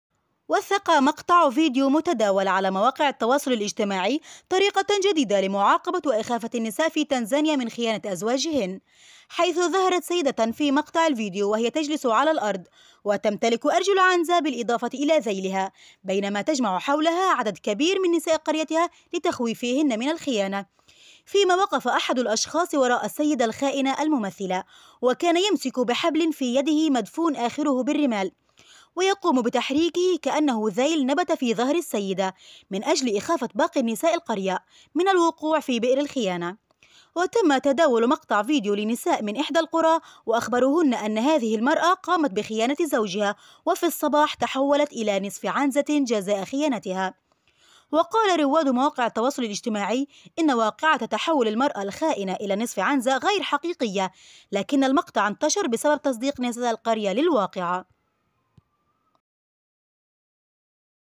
تحولت الى ماعز بعدما خـ ـانت زوجها طريقة غريبة لإخـ ـافة النساء في تنزانيا !
امرأة تحولت الى نصف  ماعز بعدما خـ ـانت زوجها طريقة غريبة لإخافة النساء في تنزانيا
وثق مقطع فيديو متداول على مواقع التواصل الاجتماعي طريقة جديدة لمعاقبة واخافة النساء في تنزانيا من خيانة ازواجهن .
🔴 حيث ظهرت سيدة في مقطع الفيديو وهي تجلس على الأرض، وتمتلك أرجل عنزة، بالإضافة إلى ذيلها، بينما تجمع حولها عدد كبير من نساء قريتها لتخويفهن من الخيانة.

🔴 فيما وقف أحد الأشخاص وراء السيدة "الخائنة الممثلة"، وكان يمسك بحبل في يده مدفون آخره بالرمال، ويقوم بتحريكه كأنه ذيل نبت في ظهر السيدة، من أجل إخافة باقي نساء القرية، من الوقوع في بئر الخيانة.

🔴 وتم تداول مقطع فيديو لنساء من إحدى القرى، وأخبروهن أن هذه المرأة قامت بخيانة زوجها، وفي الصباح تحولت إلى نصف عنزة جزاء خيانتها.

🔴 وقال رواد مواقع التواصل الاجتماعي، إن واقعة تحول المرأة الخائنة إلى نصف عنزة غير حقيقية، لكن المقطع انتشر بسبب تصديق نساء القرية للواقعة.